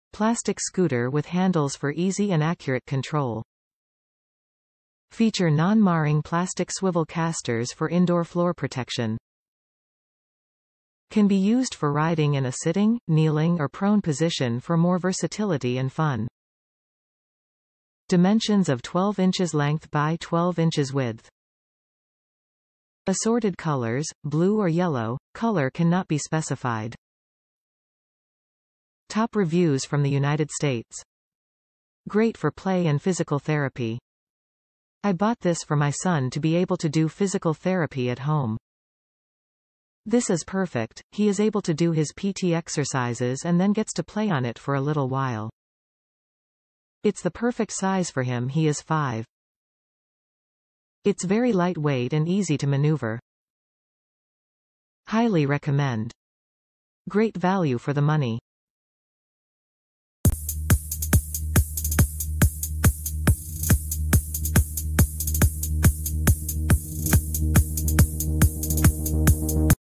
Champion Sports Standard Scooter Board with Handles, Assorted Colors (Yellow or Blue), 12 Inches
Plastic scooter with handles for easy and accurate control
Feature non-marring plastic swivel casters for indoor floor protection
Can be used for riding in a sitting, kneeling or prone position for more versatility and fun
Dimensions of 12 inches length x 12 inches width
Assorted colors - Blue or Yellow (color can not be specified)

Great for play and physical therapy
I bought this for my son to be able to do physical therapy at home. This is perfect, he is able to do his PT exercises and then gets to play on it for a little while. It’s the perfect size for him he is five. It’s very light weight and easy to maneuver. Highly recommend. Great value for the money.